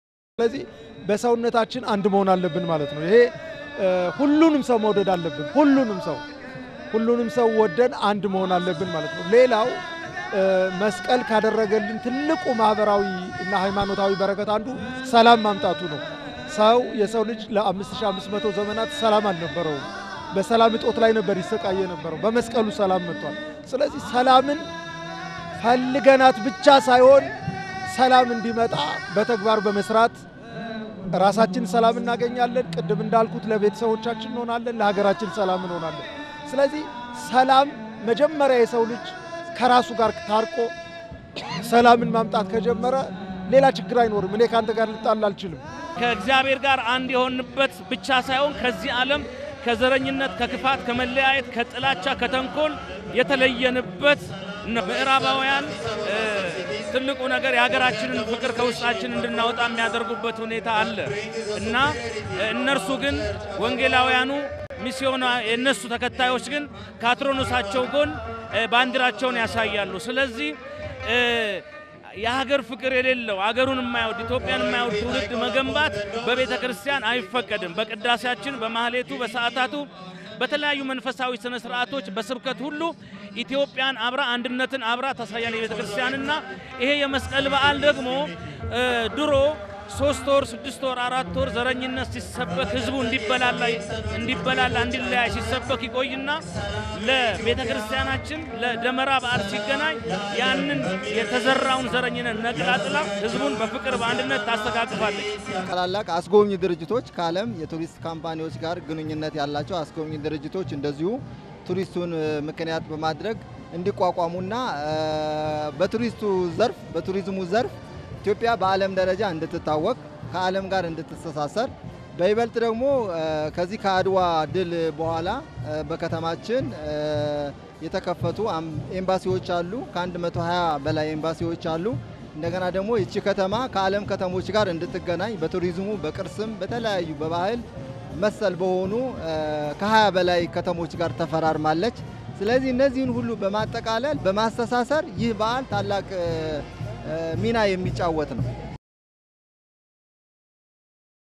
በመስቀል ደመራ በዓል ተገኝተው አስተየታቸውን ለአዲስ ሚዲያ ኔትዎርክ የሰጡ የሃይማኖት መምህራን አዲስ ሚዲያ ኔትወርክ መስከረም 17/2014 ዓ.ም
ምዕመኑ የእርቅና የፍቅር ምልክት የሆነውን የመስቀል በዓልን ሲያከብር የአንድነት የሰላምንና የመተሳሰብ እሴትን በመተግበር ሊሆን እንደሚገባ ተጠቆመ፡፡
በመስቀል ደመራ በዓል ተገኝተው አስተየታቸውን ለአዲስ ሚዲያ ኔትዎርክ የሰጡ የሃይማኖት መምህራን ኢትዮጵያ ሰላሟ ተጠብቆ አንድነቷ የተጠናከረ እንዲሆን ዜጎች ከመቼውም ጊዜ በተለየ ሊረባረቡ እንደሚገባም አንስተዋል፡፡